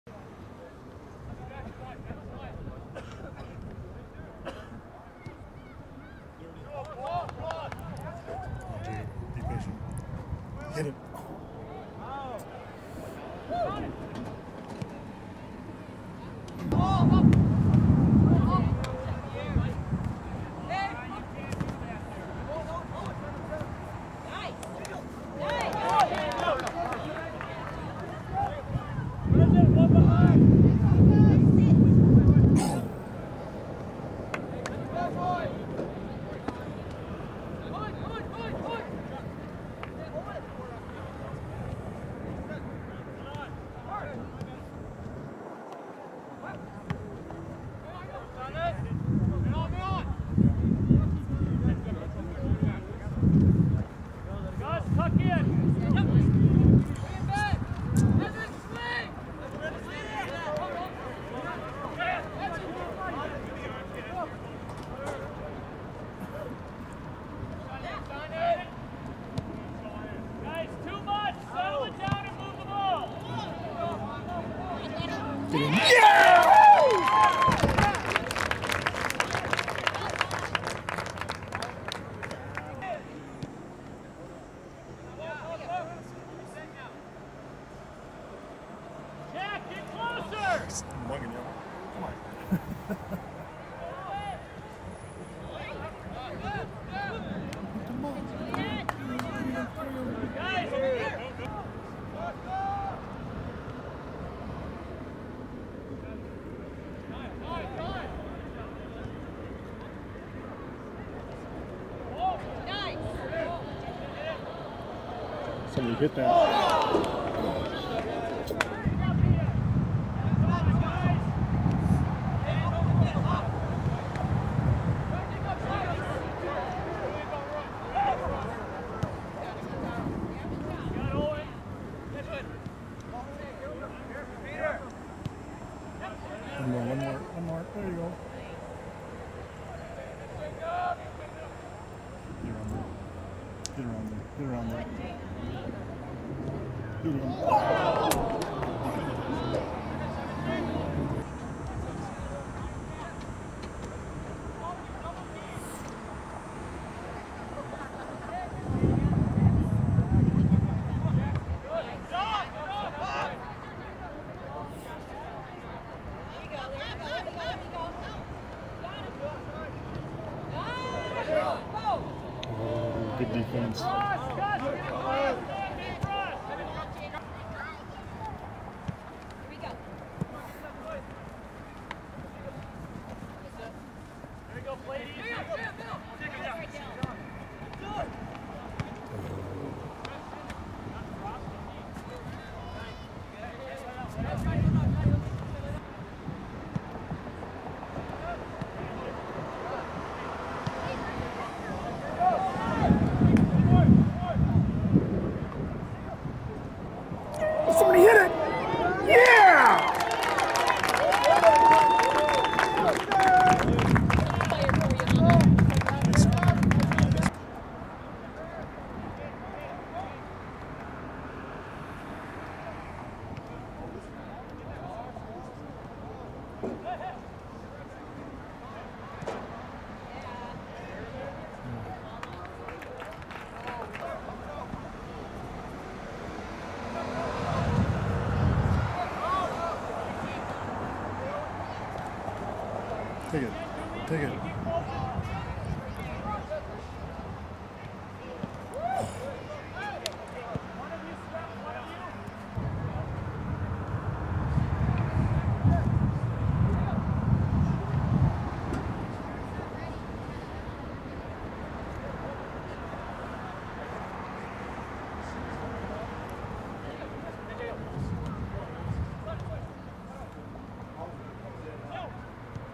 2018-09-06 EV vs Apple Valley Highlights
Highlights of Jacob from Eastview game against rivals Apple Valley.